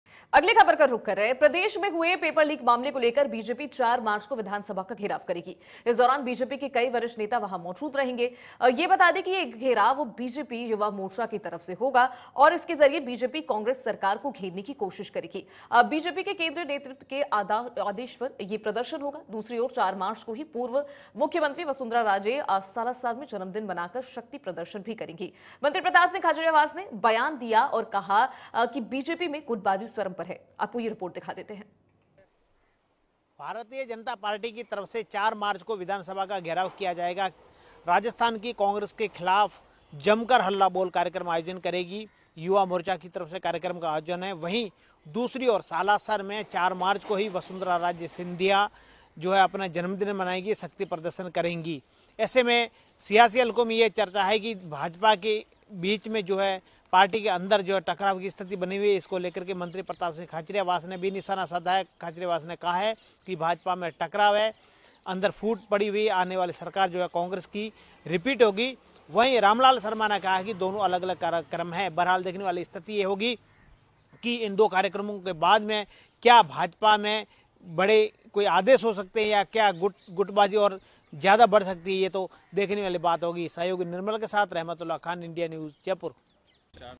Jaipur News : पेपर लीक को लेकर BJP का आंदोलन, साथ ही 4 मार्च को विधानसभा घेराव करने का फैसला
Jaipur News : पेपर लीक को लेकर  BJP का आंदोलन, साथ ही  4 मार्च को विधानसभा घेराव करने का फैसला | Hindi News